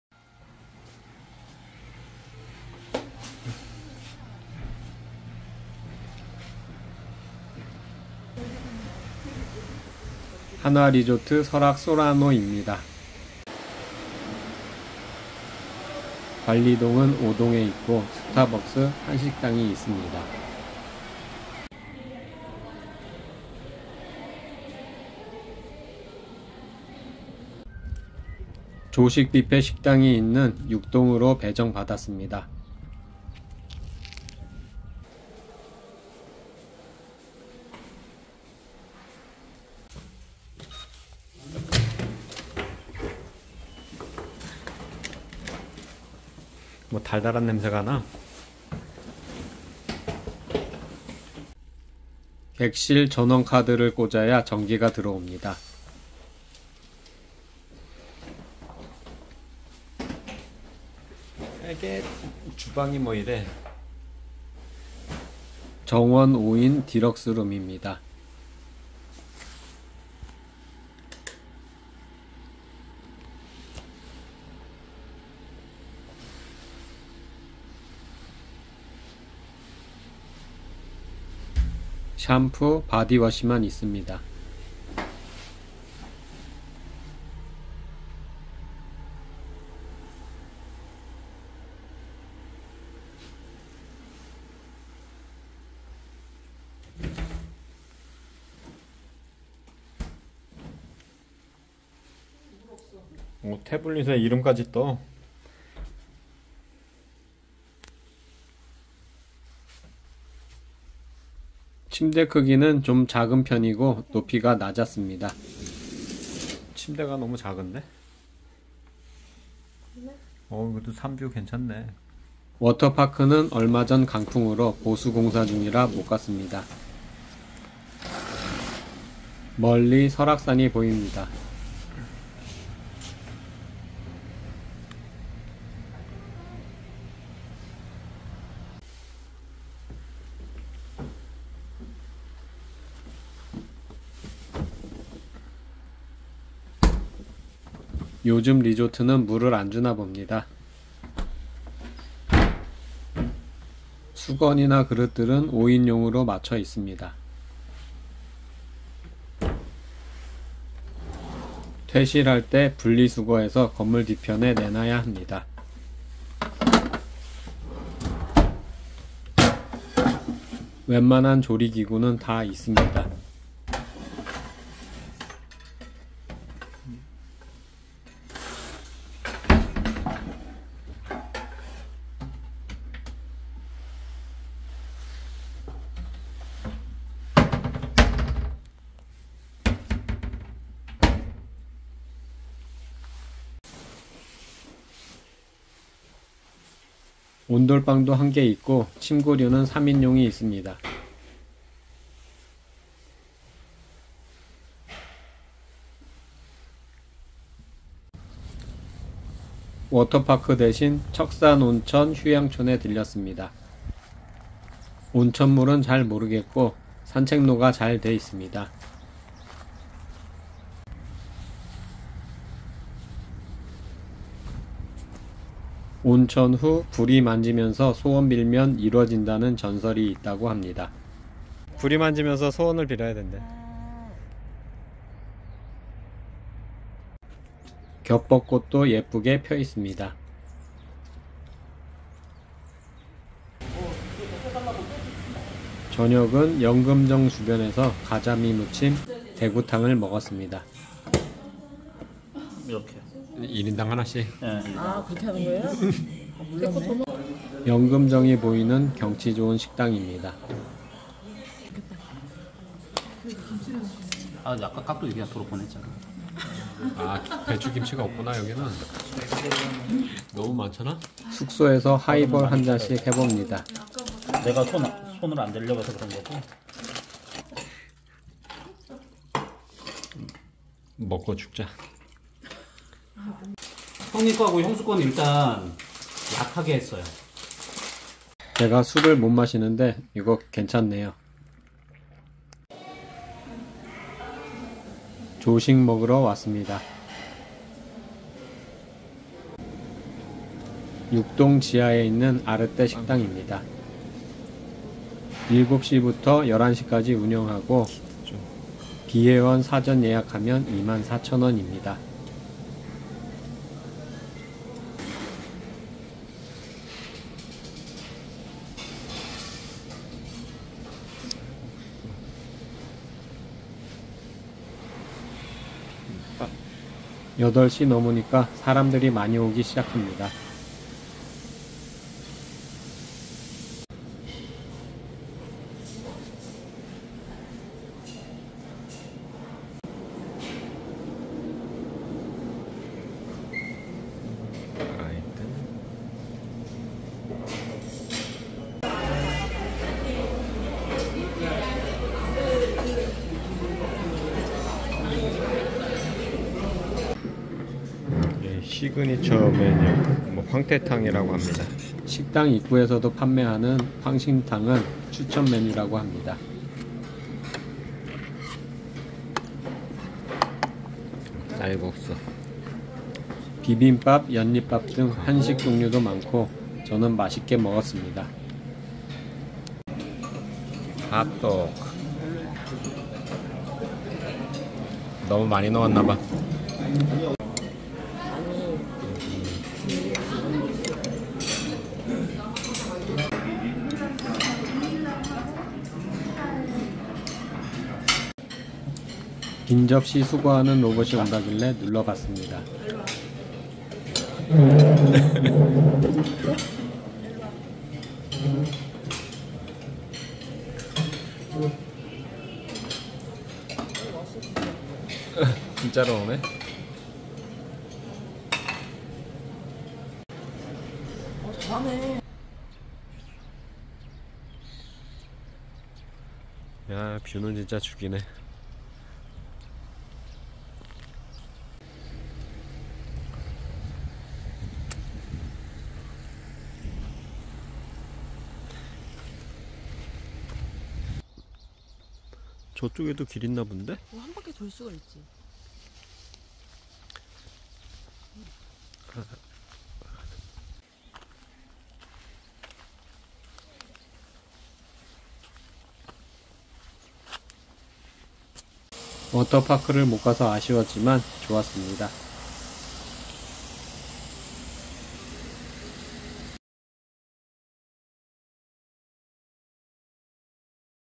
한화리조트 설악 쏘라노 디럭스 5인실, 조식뷔페
이번 속초여행중 이용했던 한화리조트 설악 쏘라노 입니다.
워터파크 패키지로 예약했었는데 강풍피해로 워터파크 수리중이라 조식뷔페로 바꿔서 예약했던 곳입니다.
설악산이 보이고 조식도 괜찮았습니다.